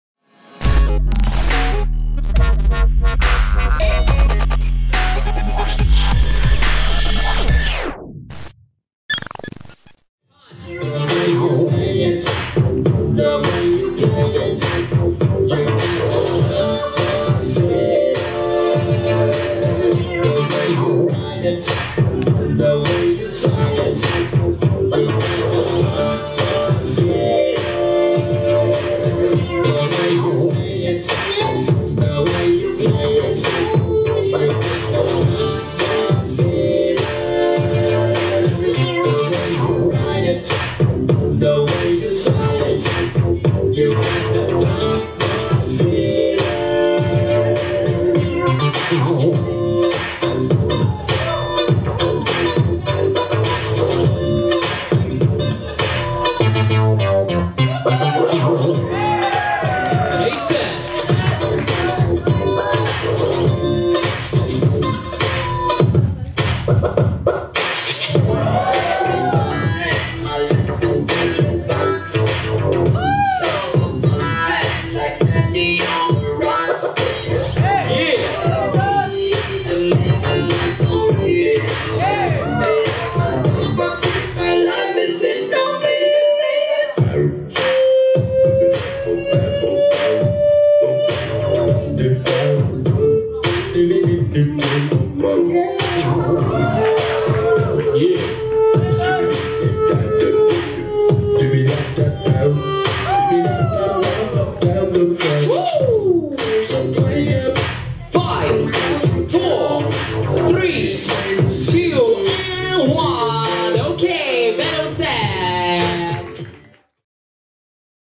Jason vs Poppin Panh | Open Cat Top8 | Pop In Progress 2015: Grand Finals | RPProductions
Pop In Progress 2015: Grand Finals
Winner: Jason